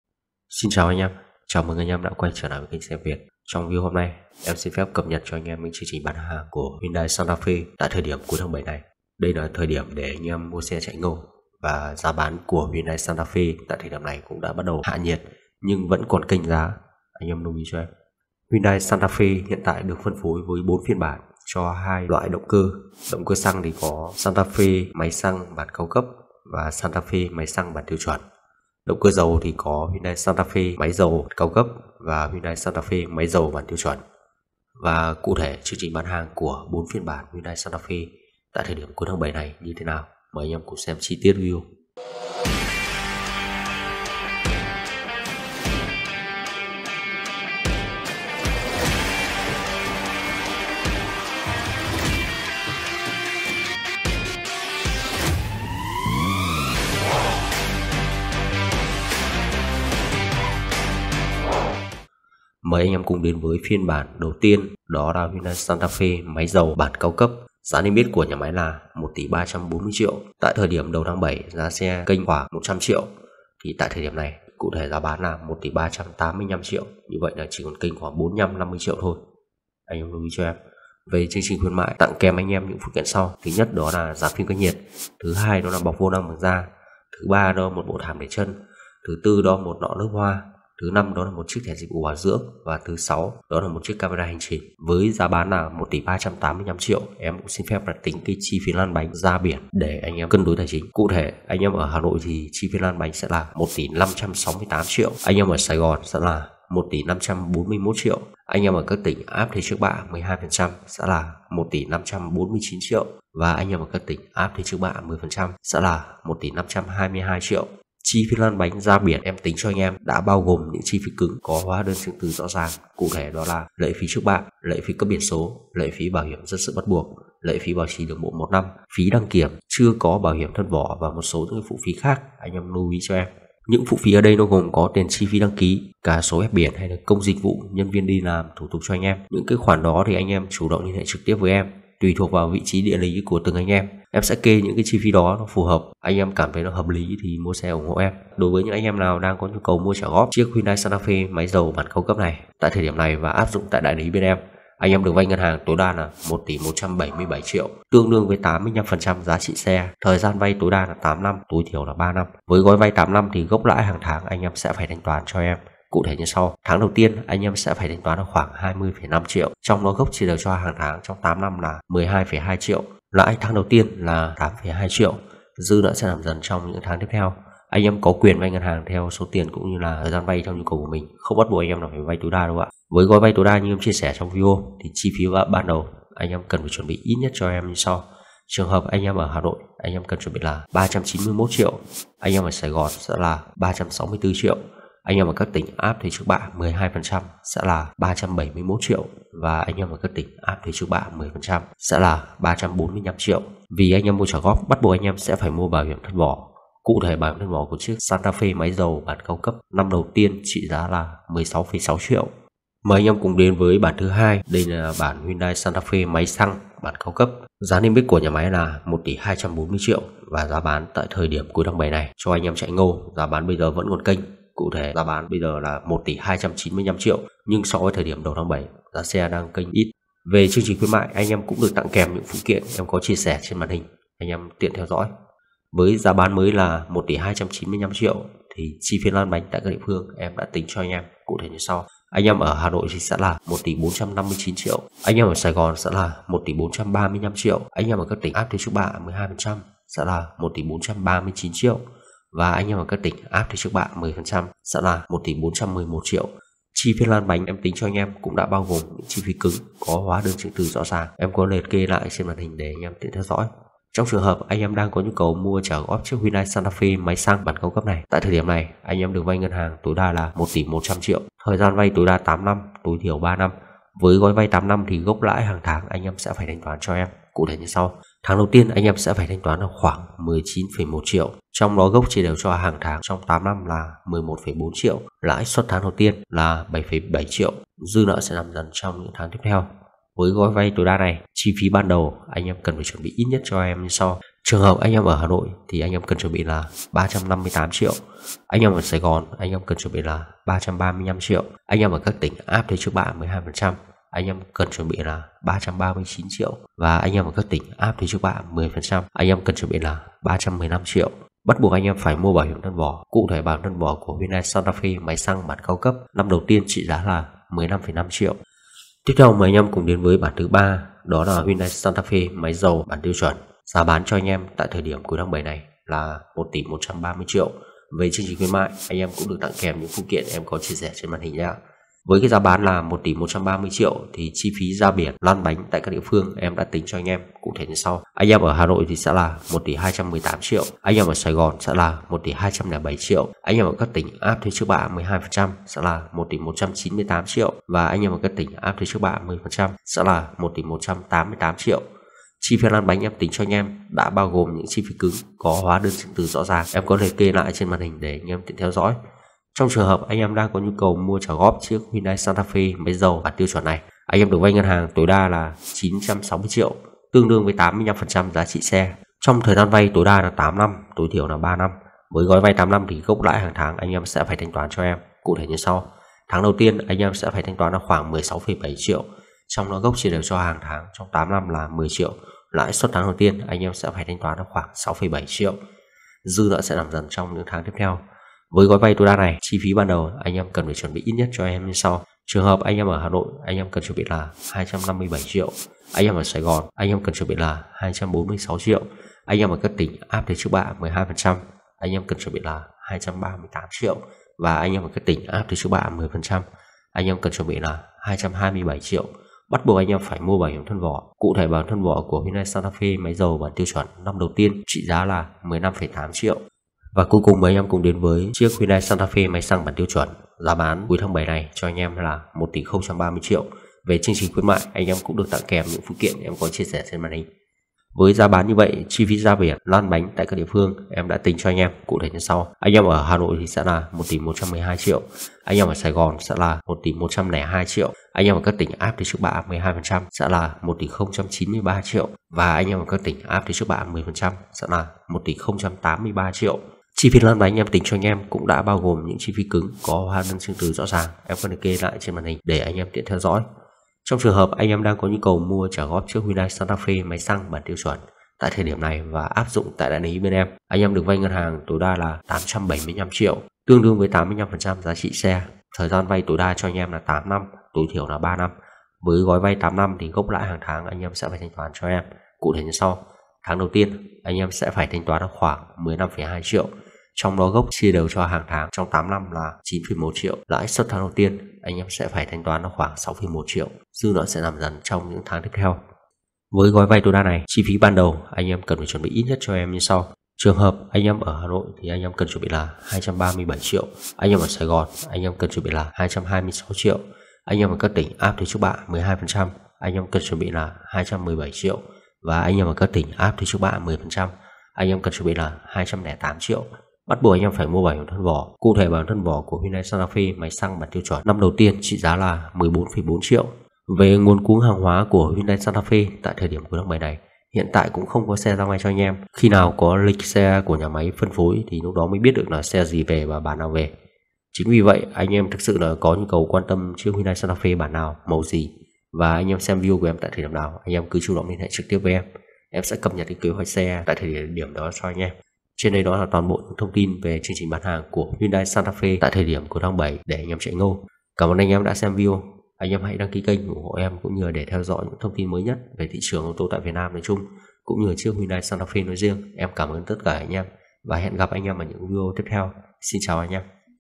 Hạ nhiệt Hyundai Santafe cuối tháng 07/2022 trước ngâu nhưng vẫn còn kèm lạc.
Cập nhật giá xe Hyundai Santafe tháng 07/2022.
Giá xe Hyundai Santafe đã hạ nhiệt nhưng vẫn còn kèm lạc.
Chi tiết lăn bánh và trả góp các phiên bản Hyundai Santafe 2022 tại các địa phương.
- Lệ phí trước bạ tại các địa phương áp như sau:
 + Áp thuế 12%: Hà Nội, Quảng Ninh, Hải Phòng, Lào Cai, Cao Bằng, Lạng Sơn
 + Áp thuế 11%: Hà Tĩnh
 + Áp thuế 10%: Các tỉnh còn lại
- Giá xe Hyundai Santafe máy xăng bản cao cấp:
  + Chi phí lăn bánh Hà Nội: 1459 triệu
  + Chi phí lăn bánh TP.HCM: 1435 triệu
  + Chi phí lăn bánh tại tỉnh áp thuế trước bạ 12%: 1439 triệu
  + Chi phí lăn bánh tại tỉnh áp thuế trước bạ 10%: 1416 triệu
- Giá xe Hyundai Santafe máy dầu bản cao cấp:
  + Chi phí lăn bánh Hà Nội: 1568 triệu
  + Chi phí lăn bánh TP.HCM: 1541 triệu
  + Chi phí lăn bánh tại tỉnh áp thuế trước bạ 12%: 1549 triệu
  + Chi phí lăn bánh tại tỉnh áp thuế trước bạ 10%: 1522 triệu.
- Giá xe Hyundai Santafe máy xăng bản tiêu chuẩn:
  + Chi phí lăn bánh Hà Nội: 1112 triệu
  + Chi phí lăn bánh TP.HCM: 1102 triệu
  + Chi phí lăn bánh tại tỉnh áp thuế trước bạ 12%: 1093 triệu
  + Chi phí lăn bánh tại tỉnh áp thuế trước bạ 10%: 1083 triệu
- Giá xe Hyundai Santafe máy dầu bản tiêu chuẩn:
  + Chi phí lăn bánh Hà Nội: 1218 triệu
  + Chi phí lăn bánh TP.HCM: 1207 triệu
  + Chi phí lăn bánh tại tỉnh áp thuế trước bạ 12%: 1198 triệu
  + Chi phí lăn bánh tại tỉnh áp thuế trước bạ 10%: 1188triệu.
Hỗ trợ trả góp 85% giá trị xe khi mua Hyundai Santafe 2022
Messenger: m.me/xeviet88